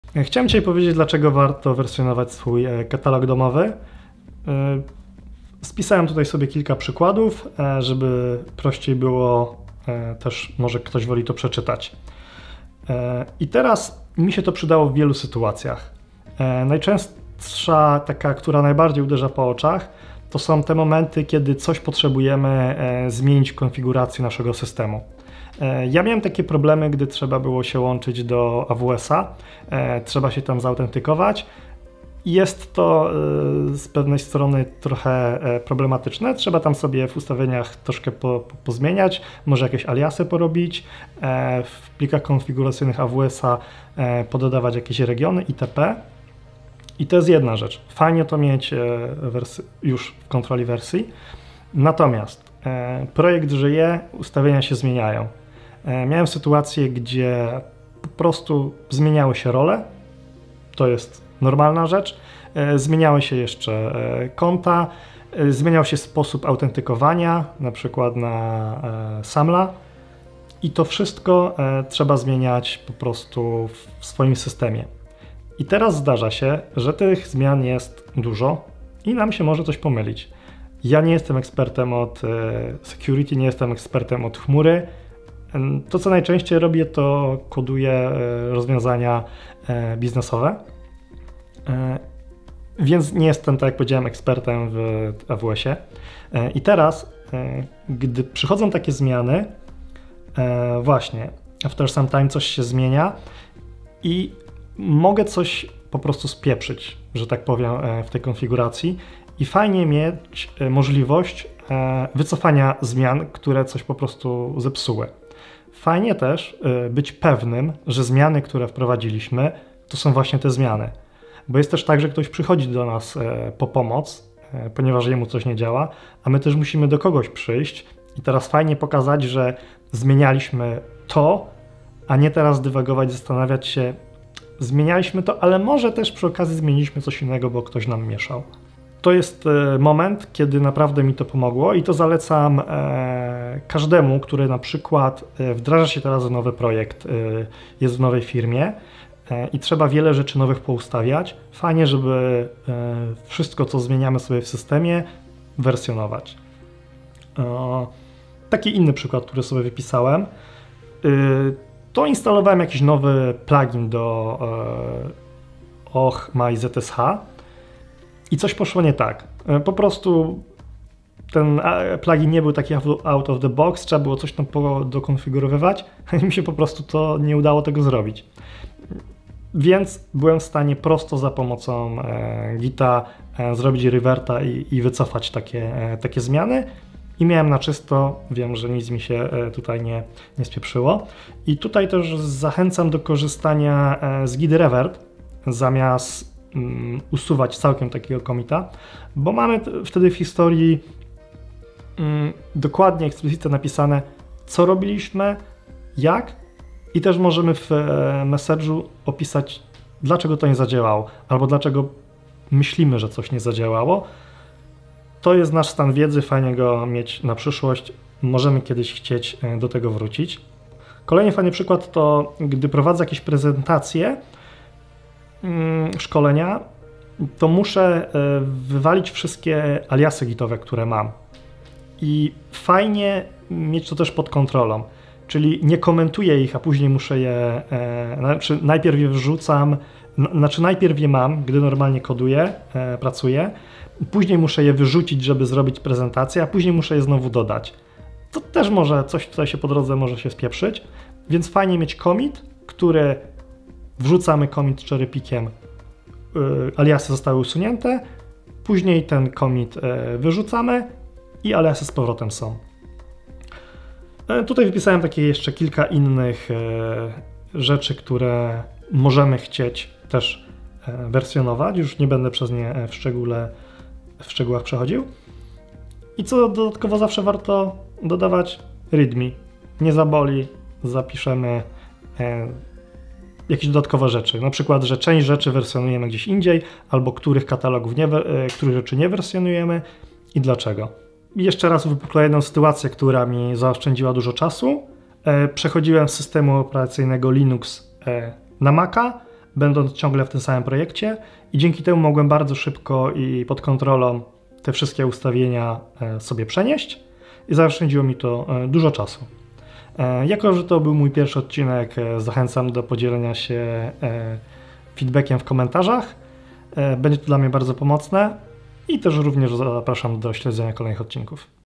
Dlaczego wrzucić ustawienia systemu do gita
Czasem setup twojego systemu jest bardziej specyficzny. Np setup dostępu do chmury AWS. A dodatkowo w trakcie trwania projektu sposoby autentykacji się zmieniają.

Warto więc te najważniejsze ustawienia trzymać w GIT, aby można wycofać zmiany gdy coś pójdzie źle i móc prosto review'ować wprowadzane zmiany.